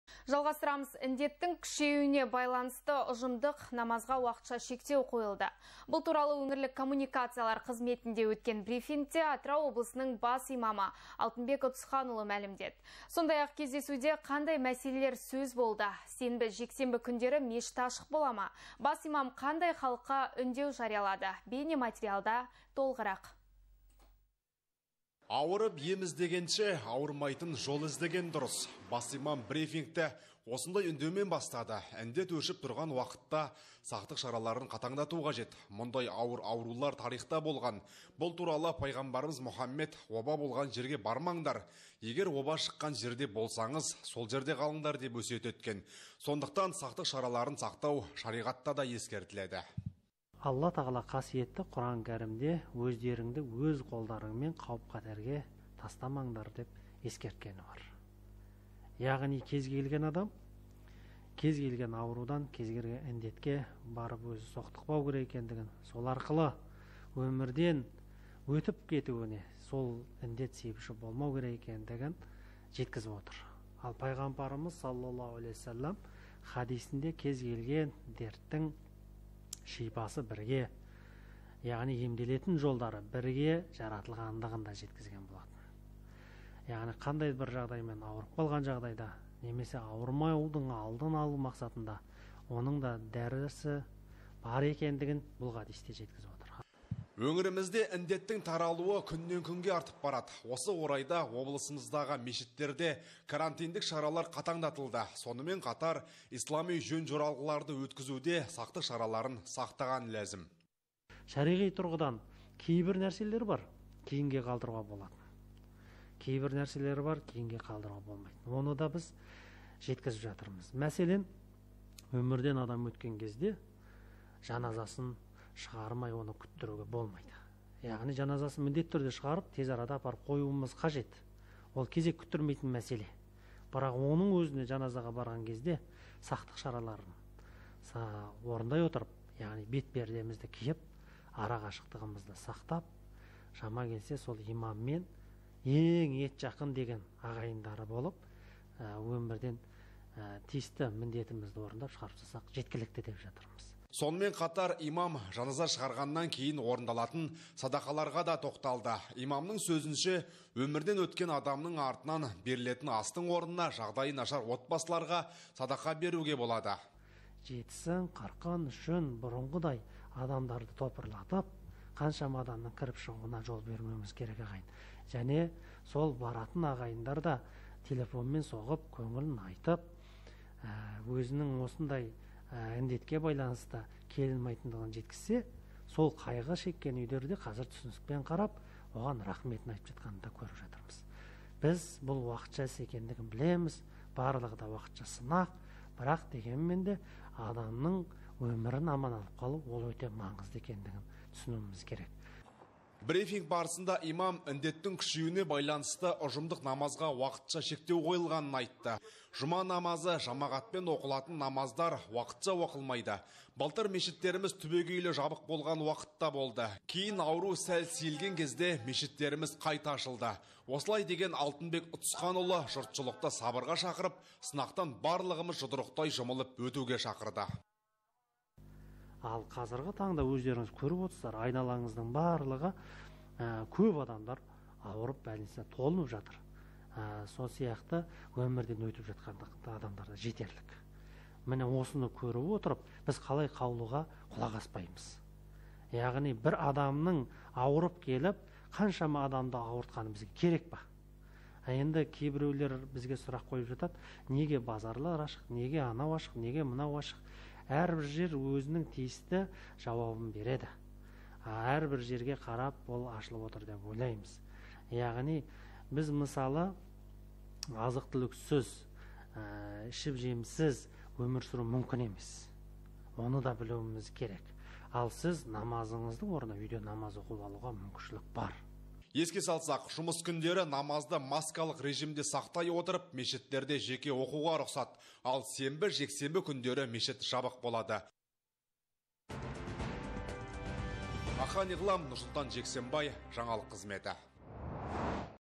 Ұжымдық намазға шектеу қойылды
Індеттің күшеюіне байланысты  ұжымдық намазға уақытша шектеу қойылды. Бұл туралы өңірлік коммуникациялар қызметінде өткен брифингте Атырау облысының бас имамы Алтынбек Ұтысханұлы мәлімдеді. Сондай-ақ, кездесуде қандай мәселелер сөз болды? Сенбі, жексенбі күндері мешіт ашық бола ма? Бас имам қандай халыққа үндеу жариялады? Бейнематериалға зер қойыңыз.